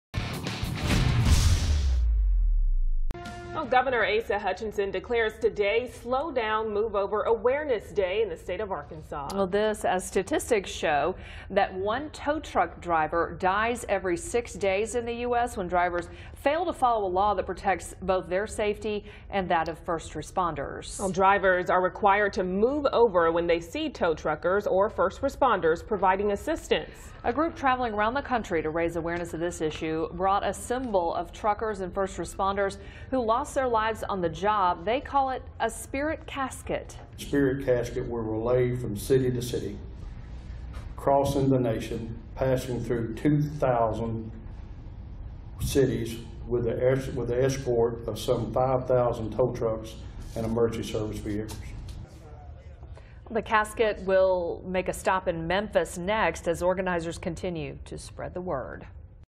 Slow Down, Move Over Awareness Day Hopes To Protect Tow Truck Drivers, First Responders KTHV
Spirit Ride: Slow Down, Move Over! Coming To A City Near You!

Contact OMG National today for all of your tow marketing needs.